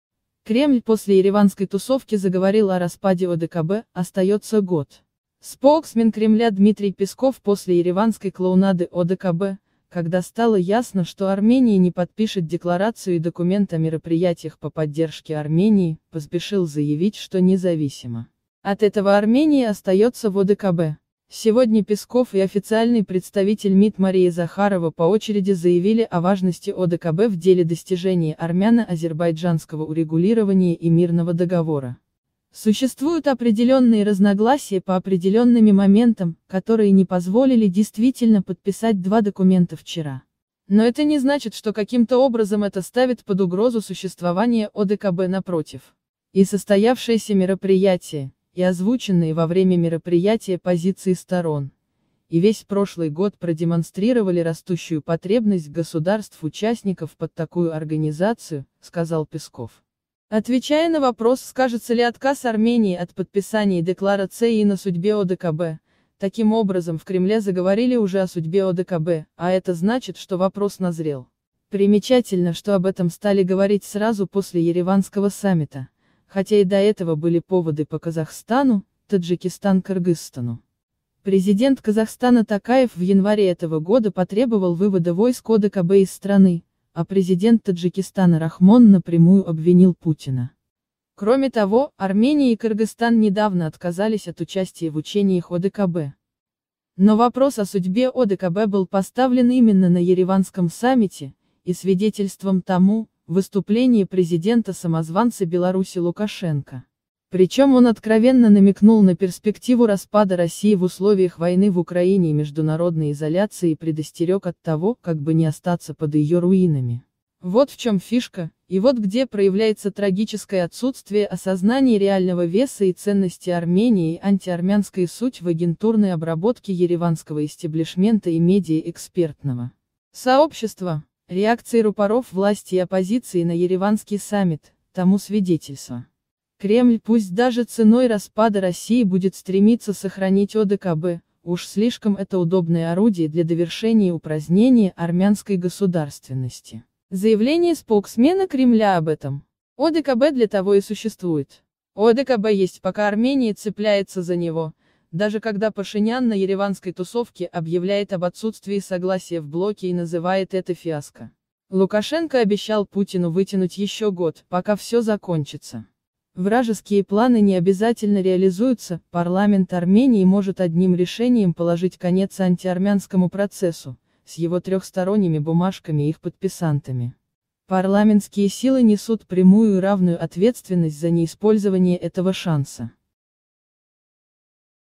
Кремль после ереванской тусовки заговорил о распаде ОДКБ: остается год.
Споуксмен Кремля Дмитрий Песков после ереванской клоунады ОДКБ, когда стало ясно, что Армения не подпишет Декларацию и документ «О мероприятиях по поддержке Армении», поспешил заявить, что независимо от этого Армения остается в ОДКБ. Сегодня Песков и официальный представитель МИД Мария Захарова по очереди заявили о важности ОДКБ в деле достижения «армяно-азербайджанского урегулирования и мирного договора».

«Существуют определённые разногласия по определёнными моментам, которые не позволили, действительно, подписать два документа вчера. Но это не значит, что каким-то образом это ставит под угрозу существование ОДКБ. Напротив. И состоявшееся мероприятие, и озвученные во время мероприятия позиции сторон, и весь прошлый год продемонстрировали растущую потребность государств-участников под такую организацию», – сказал Песков, отвечая на вопрос, скажется ли отказ Армении от подписания декларации на судьбе ОДКБ.

Таким образом, в Кремле заговорили уже о судьбе ОДКБ, а это значит, что вопрос назрел. Примечательно, что об этом стали говорить сразу после ереванского саммита, хотя и до этого были поводы по Казахстану, Таджикистан-Кыргызстану. Президент Казахстана Токаев в январе этого года потребовал вывода войск ОДКБ из страны, а президент Таджикистана Рахмон напрямую обвинил Путина. Кроме того, Армения и Кыргызстан недавно отказались от участия в учениях ОДКБ.

Но вопрос о судьбе ОДКБ был поставлен именно на ереванском саммите, и свидетельством тому – выступление президента-самозванца Беларуси Лукашенко. Причем, он откровенно намекнул на перспективу распада России в условиях войны в Украине и международной изоляции и предостерег от того, как бы не остаться под ее руинами.

Вот в чем фишка, и вот где проявляется трагическое отсутствие осознания реального веса и ценности Армении и антиармянская суть в агентурной обработке ереванского истеблишмента и медиа-экспертного сообщества. Реакции рупоров власти и «оппозиции» на ереванский саммит – тому свидетельство.

Кремль пусть даже ценой распада России будет стремиться сохранить ОДКБ – уж слишком это удобное орудие для довершения упразднения армянской государственности. Заявление споуксмена Кремля об этом. ОДКБ для того и существует.

ОДКБ есть, пока Армения цепляется за него, даже когда Пашинян на ереванской тусовке объявляет об отсутствии согласия в блоке и называет это фиаско. Лукашенко обещал Путину вытянуть еще год, пока все закончится.

Вражеские планы не обязательно реализуются, парламент Армении может одним решением положить конец антиармянскому процессу – с его трехсторонними бумажками и их подписантами. Парламентские силы несут прямую и равную ответственность за неиспользование этого шанса.
Армения
Арцах
Карабах
Азербайджан
Срочные новости

Экстренные новости
Новости дня
Վերջին լուրեր
Յութուբ լուրեր
Շտապ